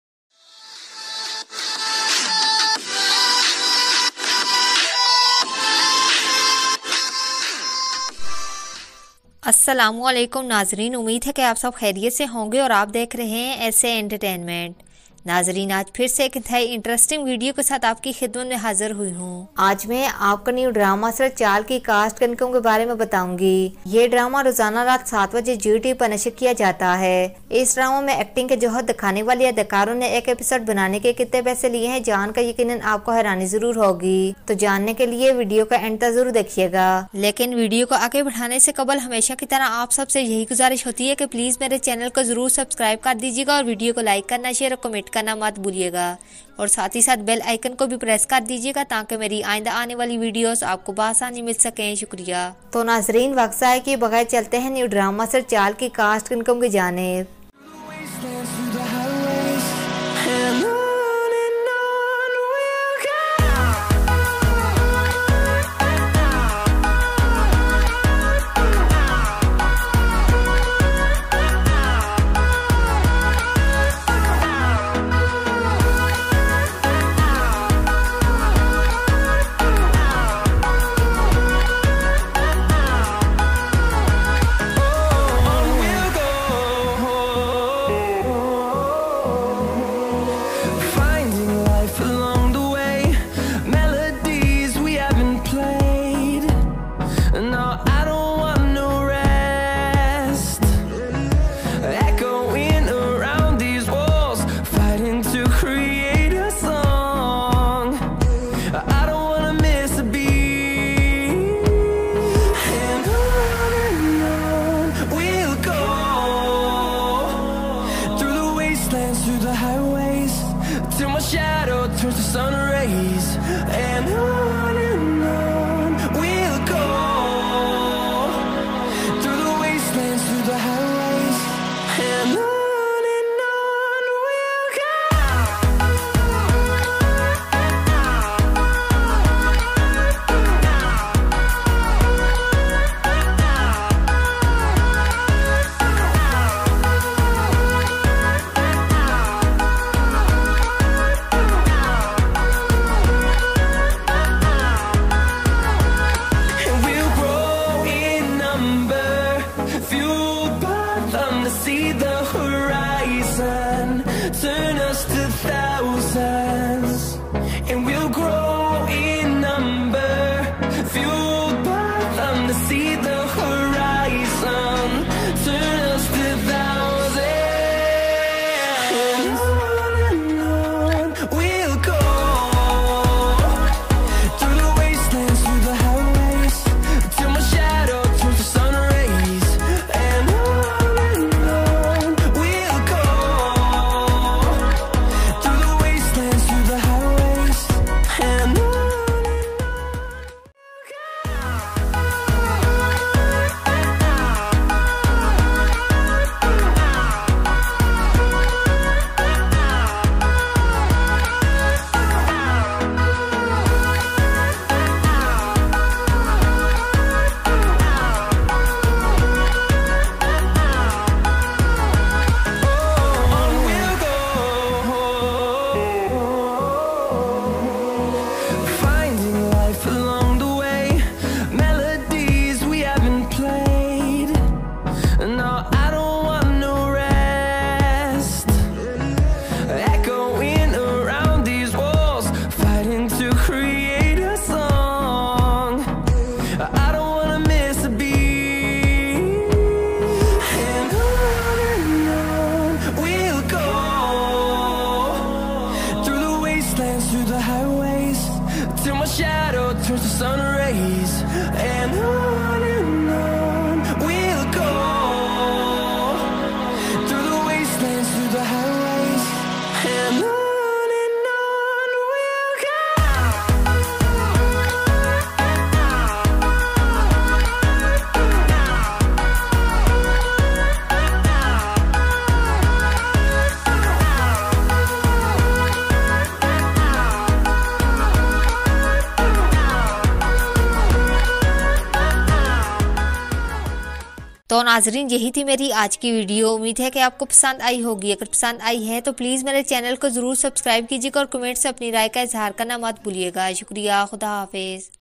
Chaal Drama Cast Salary Last Episode|Chaal All Cast Real Name Salary#Chaal
Assalamualaikum
Chaal drama cast salary. New har pal geo drama chaal all cast real names and salary. Ali ansari zubab rana and arez ahmed new drama chaal onair every Monday to Sunday tonight 7:00 pm only on har pal geo.